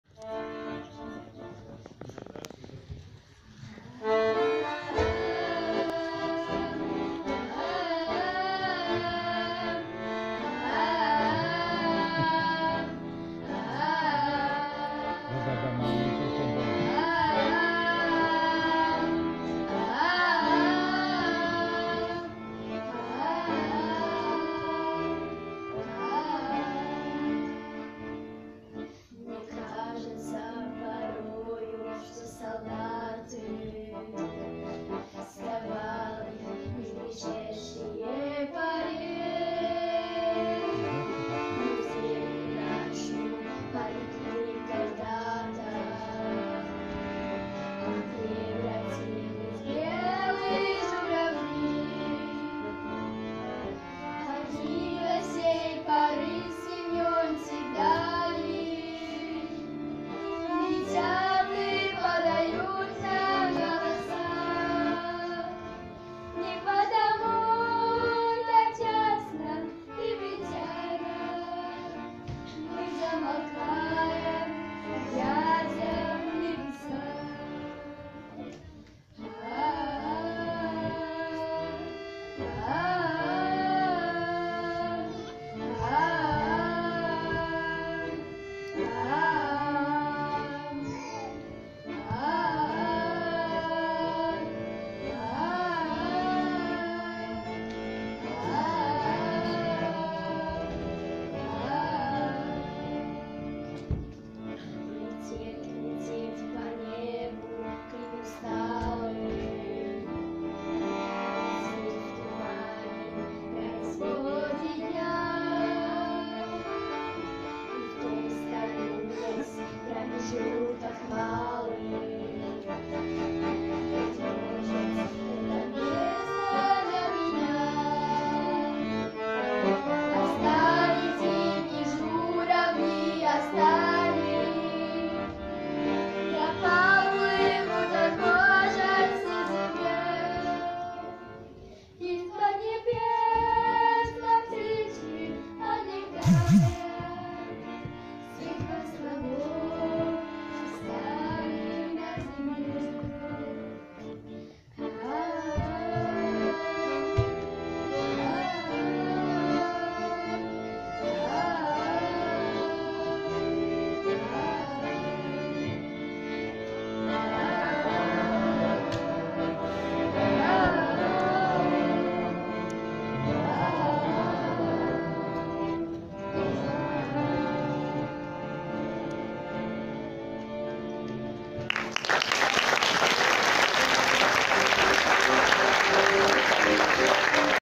Београд 2017 02 14 v6 - Србче и ЕФЕС фонд
Редовни годишњи концерт је одржан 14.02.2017. у Дому културе Чукарица, Тургењева 5, Баново Брдо.(Снимак 6)

Концерт је назван хуманитарним за помоћ манастиру...
А већ је помогнуто и неколико породица.